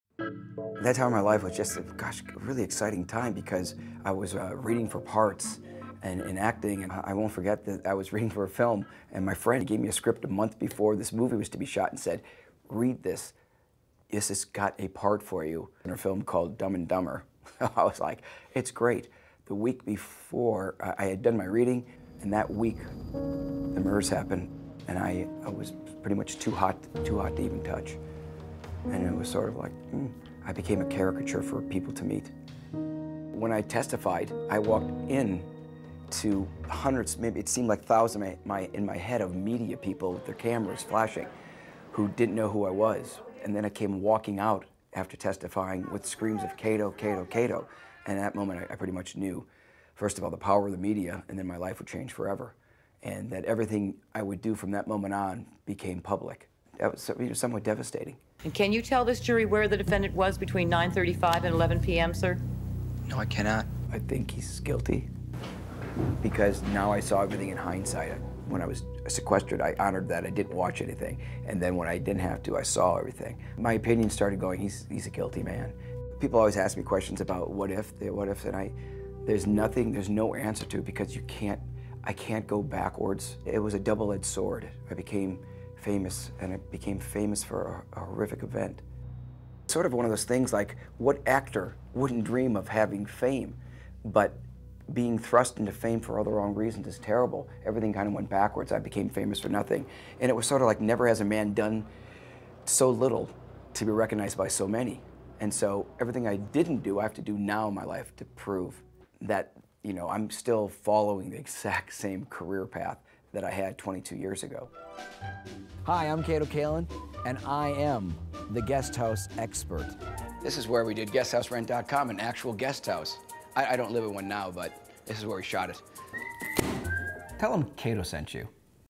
Kato Kaelin on becoming famous for being O.J. Simpson's houseguest
Kato Kaelin, an aspiring actor who had been living in O.J. Simpson's guesthouse at the time of the Nicole Brown Simpson and Ron Goldman murders, describes how his life was changed by his unexpected involvement in "the trial of the century."